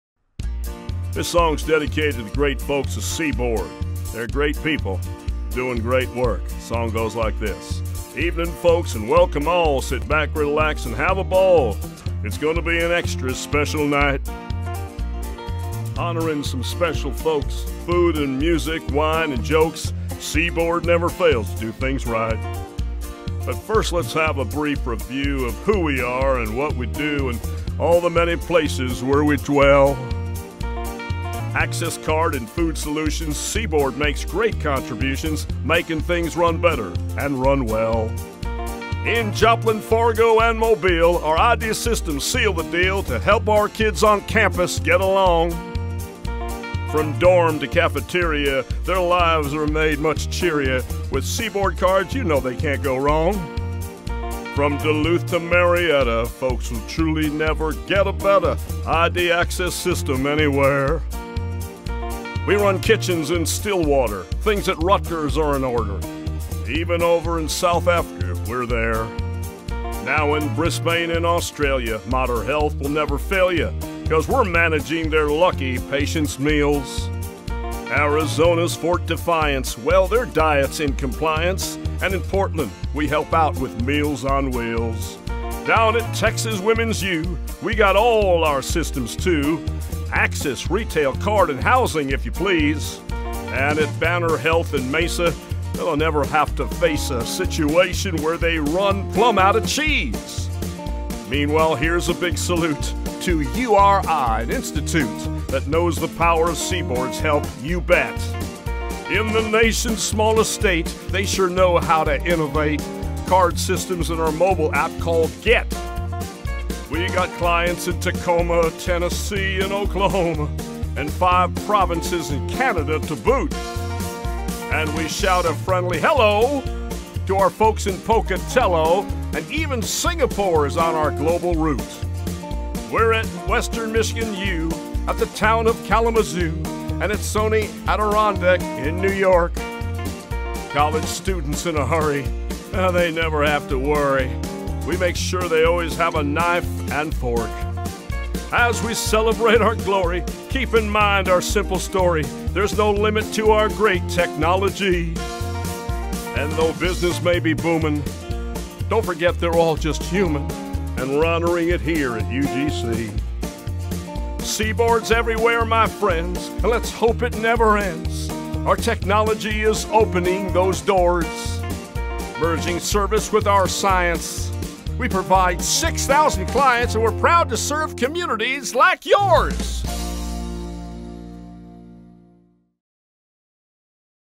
Celebrating Excellence at UGC 2018 by Roy Firestone
Our host for the 2018 Excellence Awards dinner was seven-time Emmy Award-winning interviewer, actor, writer, and entertainer, Roy Firestone. You may remember him from that scene in Jerry Maguire where he made Cuba Gooding Jr. cry. Roy created an original video tribute to CBORD and our customers (with a little help from the marketing team) which he performed live on stage for us. It was great to hear pockets of cheers from the crowd as each city or organization was named! We hope you will enjoy this short video as much as we did at UGC.